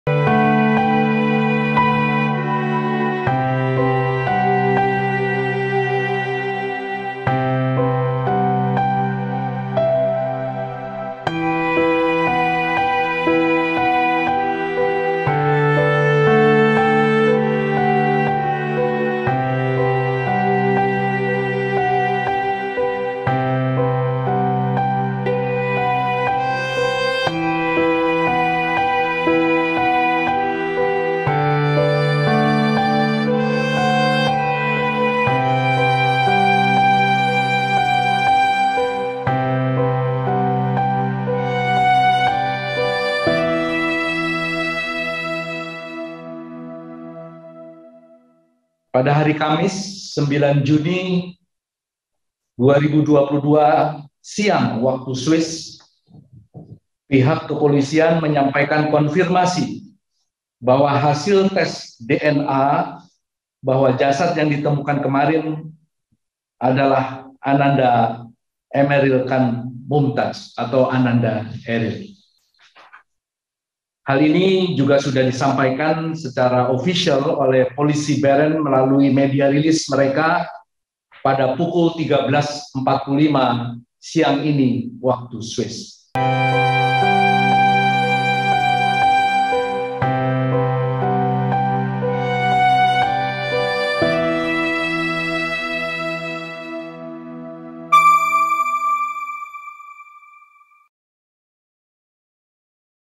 Ridwan Kamil Mandikan Jenazah Eril, Kondisinya Utuh dan Wangi
Setelah 12 hari pencarian, jenazah Emmeril Kahn Mumtadz atau Eril akhirnya ditemukan. Hal ini dikonfirmasi saat pihak Kepolisian Bern melakukan tes DNA terhadap jasad yang ditemukan pada Rabu (8/9) pagi.

Android: inews.id/android
iPhone: inews.id/ios